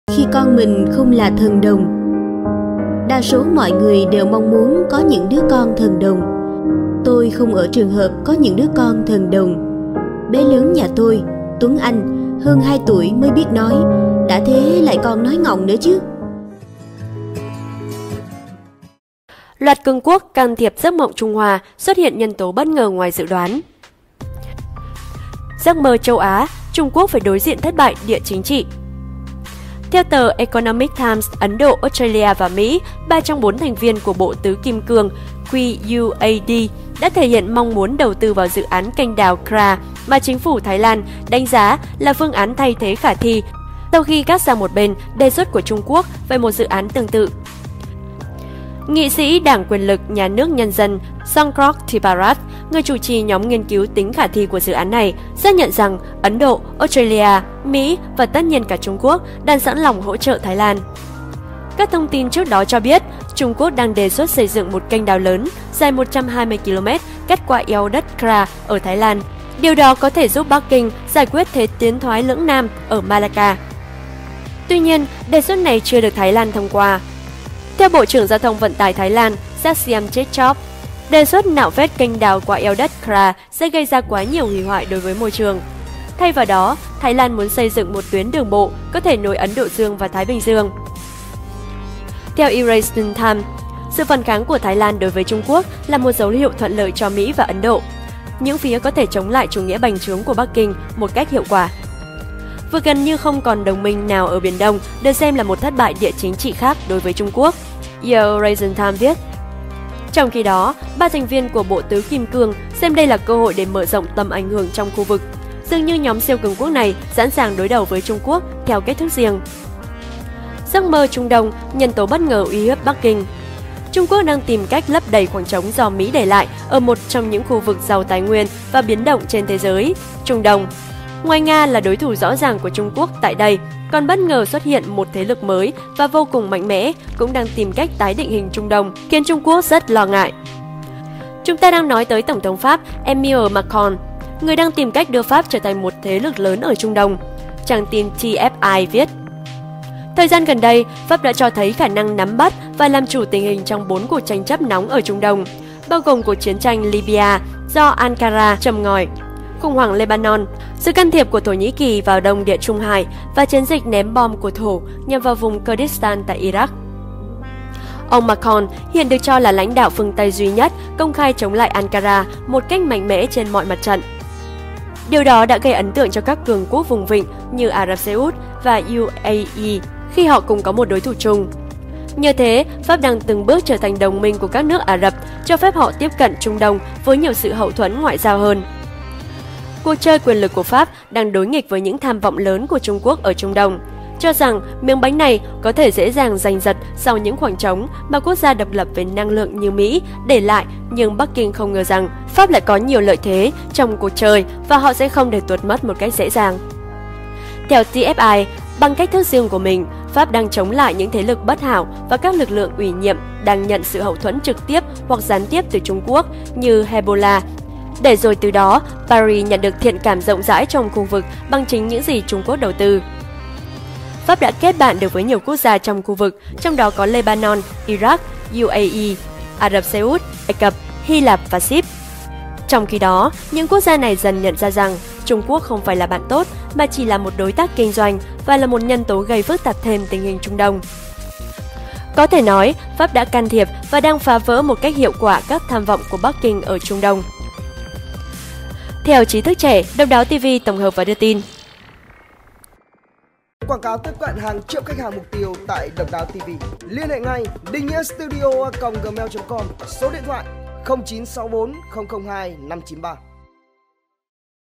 Trung Quốc & Giấc Mơ Trung Đông: Nhân Tố Bát Ngờ 'Uy Hiếp' Bắc Kinh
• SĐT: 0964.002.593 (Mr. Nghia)

Theo tờ Economic Times, Ấn Độ, Australia và Mỹ - 3 trong 4 thành viên của Bộ tứ kim cương (QUAD) đã thể hiện mong muốn đầu tư vào dự án Kênh đào Kra mà chính phủ Thái Lan đánh giá là phương án thay thế khả thi sau khi gác sang một bên đề xuất của Trung Quốc về một dự án tương tự.

Nghị sĩ Đảng Quyền lực nhà nước nhân dân (PPRP) Songklod Thipparat – người chủ trì nhóm nghiên cứu tính khả thi của dự án này – xác nhận rằng Ấn Độ, Australia, Mỹ và tất nhiên cả Trung Quốc, đang sẵn lòng hỗ trợ Thái Lan.